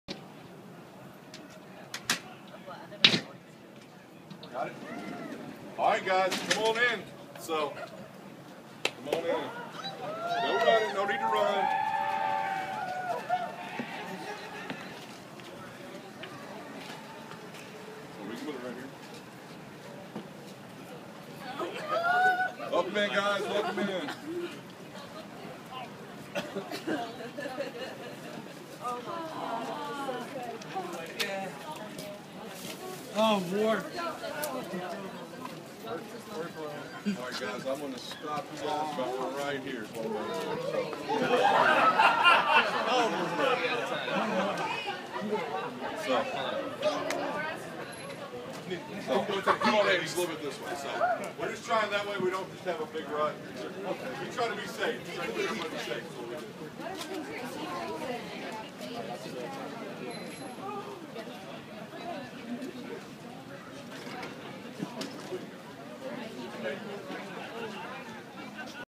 Best Buy 280 doors opening at Midnight crowd cheers
Black Friday 2012